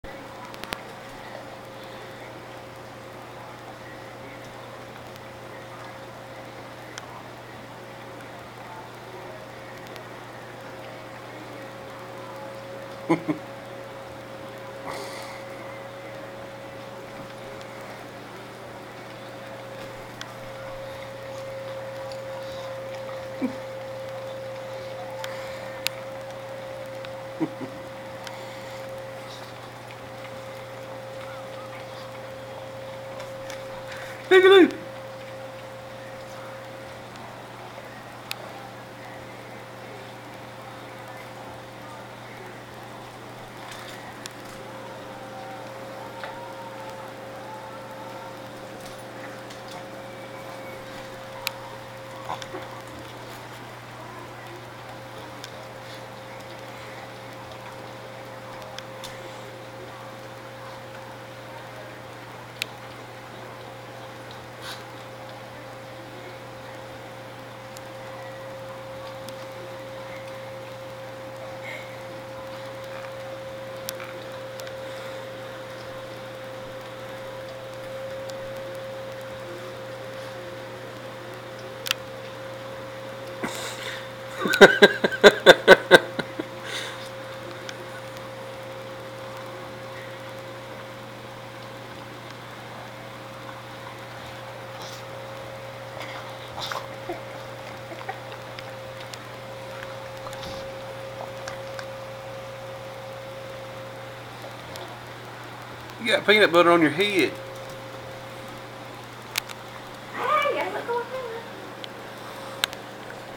Duke and Luke eating peanut butter
Duke and Luke enjoying a snack!